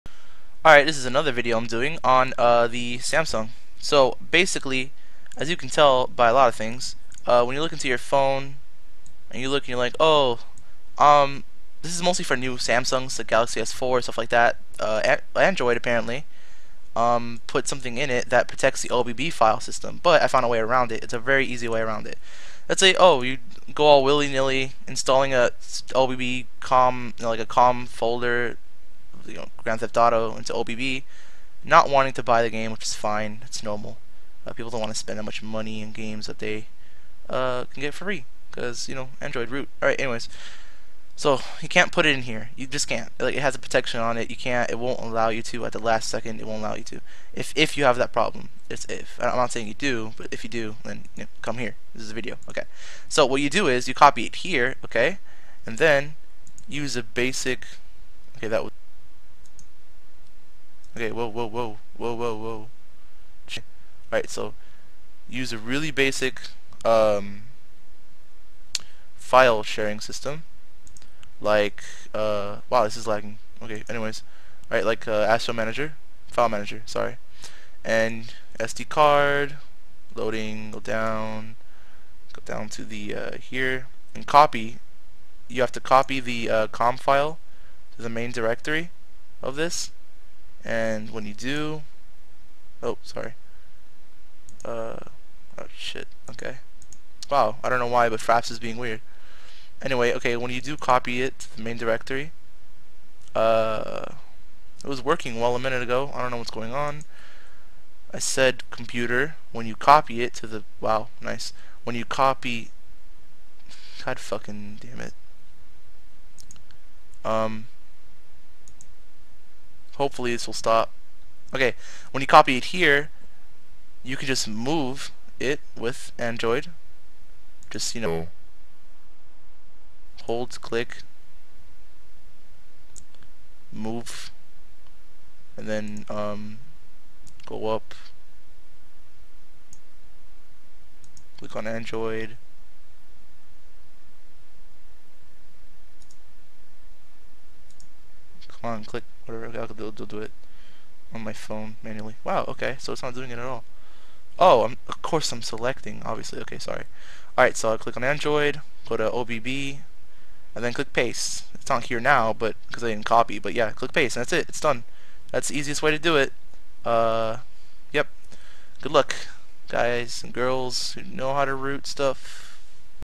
A fix for newer android devices, if you can't copy anything into the OBB directory
This is a work-around for the issue
"Cannot copy item the device has either stopped responding or has been disconnected."

It's a very easy work-around.